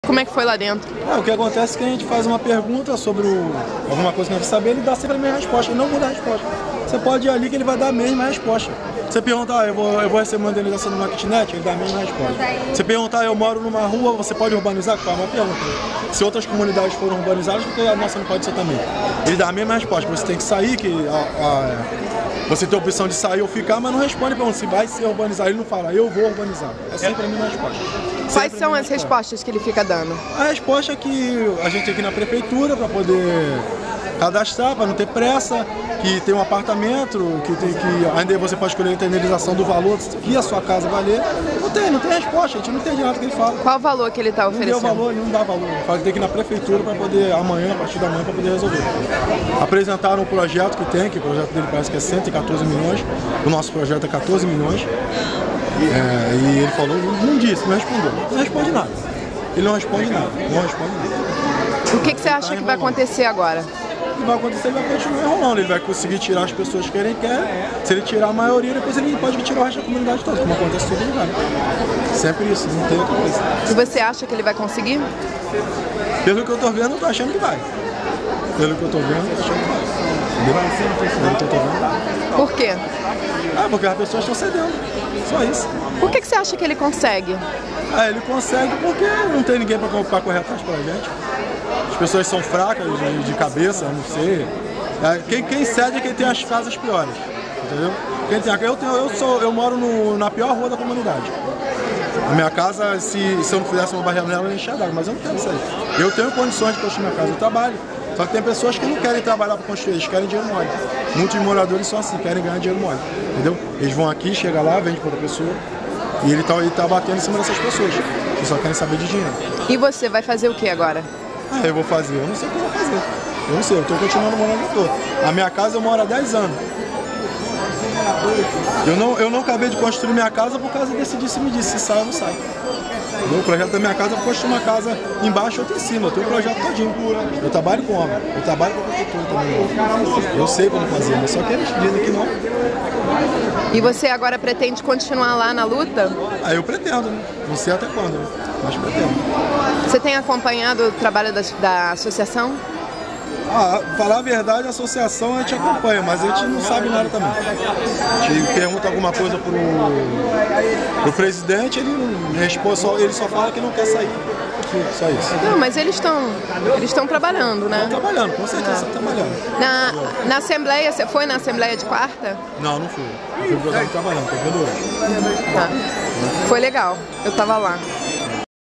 Morador da Vila Autódromo conta porque saiu cedo de reunião com o Prefeito
6 de outubro de 2013, no Riocentro--Um grupo seleto de moradores da Vila Autódromo foram convidados para participar de uma reunião com o Prefeito Eduardo Paes, porém diversos moradores foram, e finalmente, após forçar entrada em vários portões, conseguiram participar. Aqui um morador fala de sua frustração após pedir informações sobre a urbanização da comunidade na reunião.